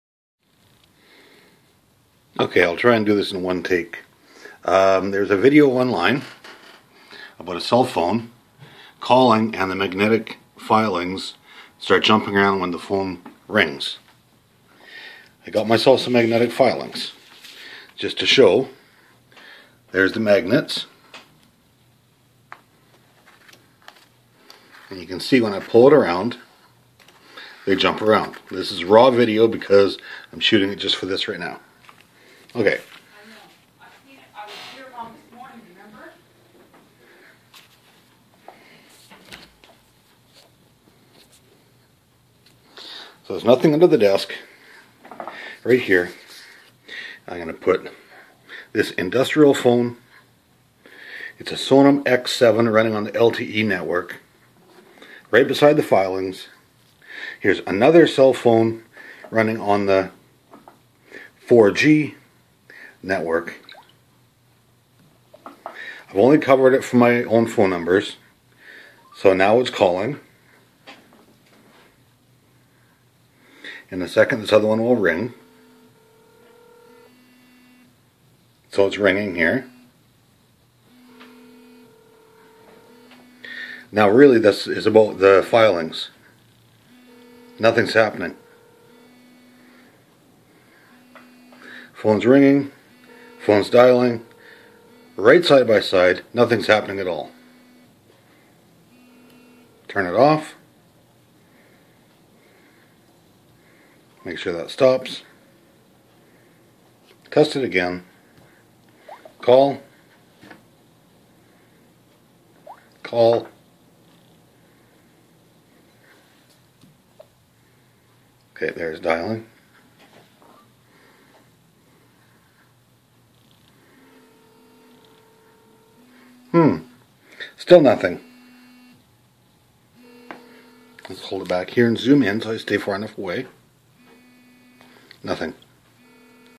Cell phones VS Iron Filings.
This is in response to a video I saw of cellphones causing iron filings to dance and move. Here's my evidence.